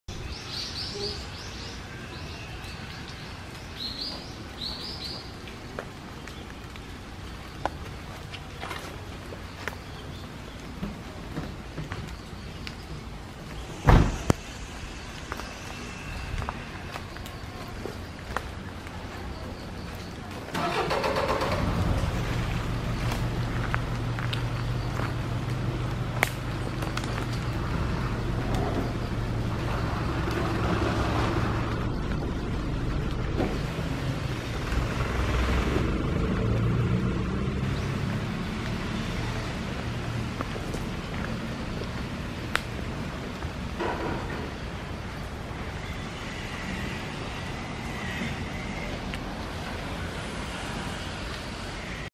高知市保護樹林 長浜嶋宮神社の森
高知市保護樹林（21）　嶋宮神社の森は新川川の河口で長浜4976番地広さ400㎡　昔は浦戸湾内に玉島　狭島　嶋宮を三島と呼び三女神をお祭りしていたらしい、境内には根元に近い場所から二本に分かれた大きなクスノキがあり　入口には大蛇の様な変わった藤の木？がある。